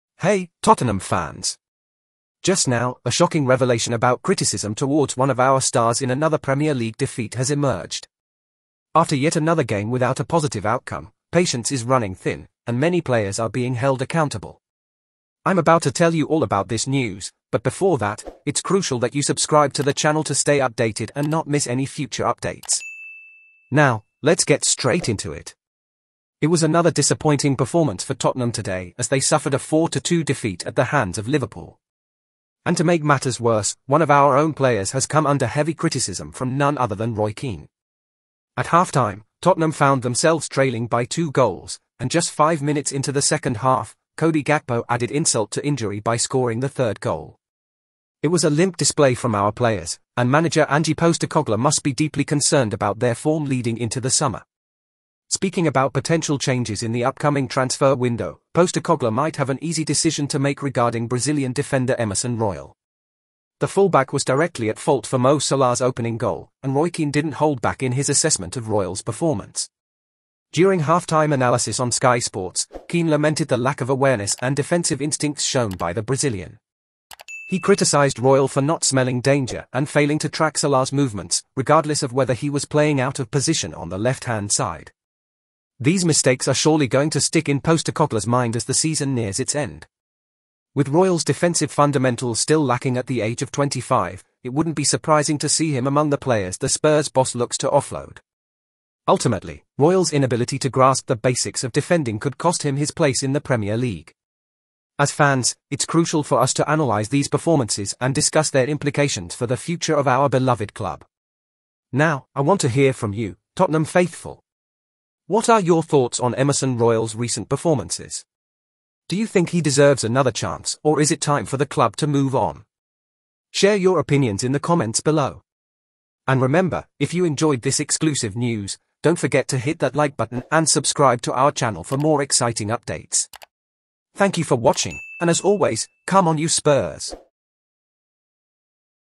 ⛔🤯LEFT NOW! SHOCKING REVELATION! PATIENCE HAS RUN OUT! TOTTENHAM LATEST NEWS! SPURS LATEST NEWS!
⚽ Hey, Tottenham fans! Just now, a shocking revelation about criticism towards one of our stars in another Premier League defeat has emerged! After yet another game without a positive outcome, patience is running thin, and many players are being held accountable!

📰 News Credits: tbrfootball

Reuters